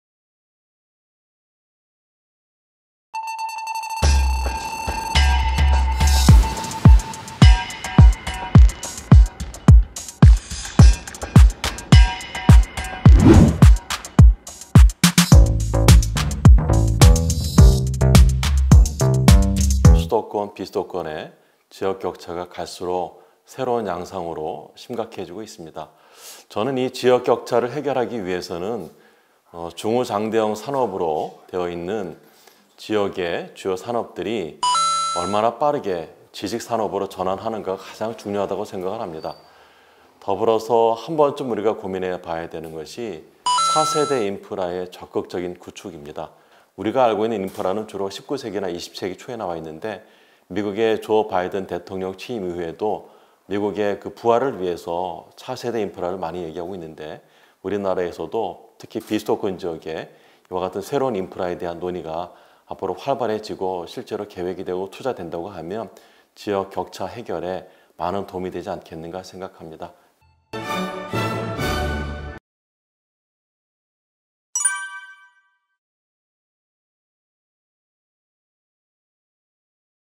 tvU 정책제언 “도시전문가에게 집값을 묻다”(7)
월간 지방정부·인터넷신문tvU- 국토도시학회
주택문제 해결을 위한 정책 제언

7편. 집값을 잡으려면 ... 지역산업, 지식산업으로 전환하라

산업구조가 바뀌고 인구구조가 바뀌면서 서울-수도권, 비수도권 사이의 격차가 갈수록 새로운 양상으로 심각해지고 있습니다. 이 문제를 해결하기 위해서 가장 중요한 전략은 비수도권 주축을 이루고 있는 중후장대형 산업을 얼마나 빠르고 얼마나 효과적으로 지식산업으로 전환하는가에 달려 있을 것 같습니다.
- 이영성 서울대 환경대학원 교수